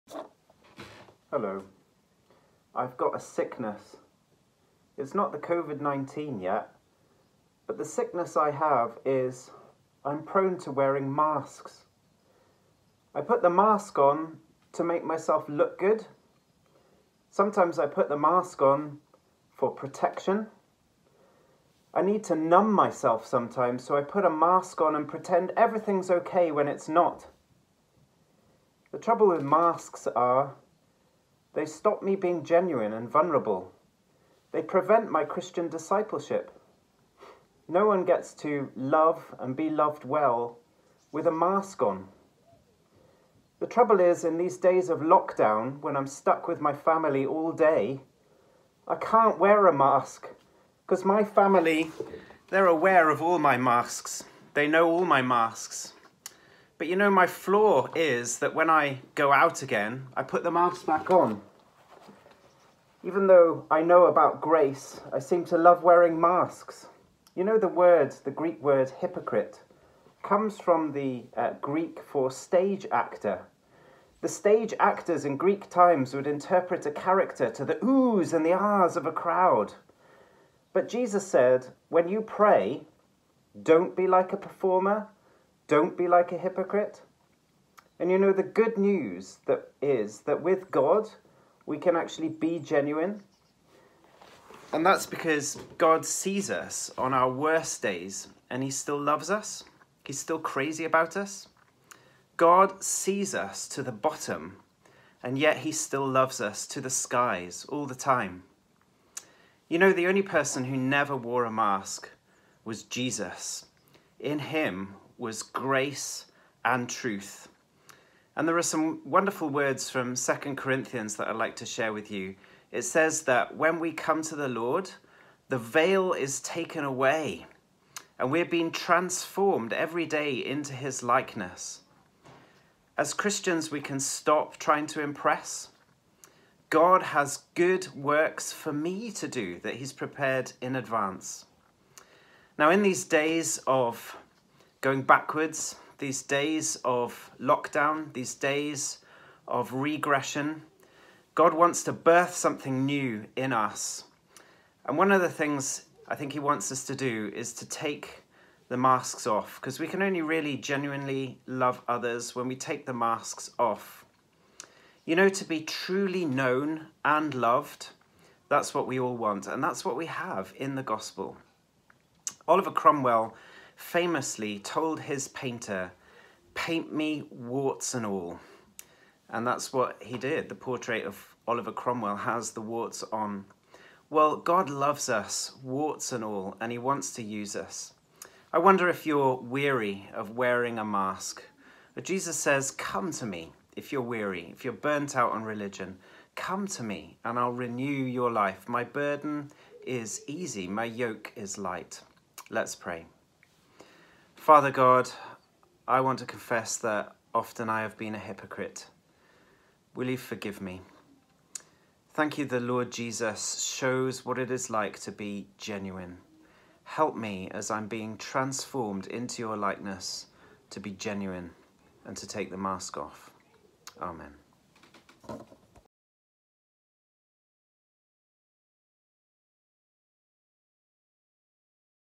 Daily Devotion // Tuesday 31st March // Chris Webb
Chris Webb brings us today's devotion - with a special guest...

Transcript below:
I have a sickness. Not Covid 19 yet.

My sickness is I am prone to wearing masks.

I wear them to hide my true self. I wear them to protect myself. I wear them to try to look good in front of others. The trouble with the masks I wear is, they stop me being genuine and vulnerable. Wearing masks prevents my healing and my Christian discipleship. Because No-one gets to love or be loved well in self protection.

Trouble is I can’t wear masks very well in front of my family. My family are very familiar with my masks and they see right through them. I’m  forced in these days of lockdown to be maskless and its painful!

The word hypocrite comes from a Greek term for stage actor; someone who was highly skilled in wearing a physical mask and interpreting a certain character to others. They would hone their craft in front of crowds of people to the oohs and aahs of the adoring public who were completely taken in with their portrayals.

Jesus says: When you pray, do not be like the hypocrites. For they love to stand and pray in the synagogues and street corners so they might be seen by others. Truly they have received their reward.

Praying like a Greek performer might work with a crowd, wearing a disguise and putting on a show and getting some likes and retweets but it doesn’t work with the one who sees all things straight into our soul. Have you ever found yourself trying to impress a God who you imagined was growing more and more impatient with you?

But good news is, With God we can be genuine. God sees us on our worst days and he is still crazy about us.

 But whenever anyone turns to the Lord, the veil is taken away. 17 Now the Lord is the Spirit, and where the Spirit of the Lord is, there is freedom. 18 And we all, who with unveiled faces contemplate the Lord’s glory, are being transformed into his image with ever-increasing glory, which comes from the Lord, who is the Spirit.

When we become children of God, we enter a house where we have true freedom. The kind of freedom where we can take off the masks we put on to try and impress other people and to try to win the reward of their affection, approval or friendship.

To the extent that we believe this, we can start to take off the masks and be genuine with others.

 To be fully known and truly loved is what we need more than anything. It liberates us from pretense, humbles us out of our self righteousness and fortifies us for any difficulty life can throw at us.

Oliver Cromwell famously asked the man painting his portrait to paint him warts and all. God loves us, with all our warts. We can try covering our warts over in all manner of ways but eventually they do reveal themselves.

Are you tired of pretending. Weary of being weary. There is a room of grace where God and broken sinners can live together. Come and join us. You're welcome here.

Prayer:

Father, I confess that I have been a hypocrite. I confess that my heart craves attention and approval and I so often seek it in the wrong places. Please forgive me and grant me grace to remove the masks I wear. In Jesus name. Amen.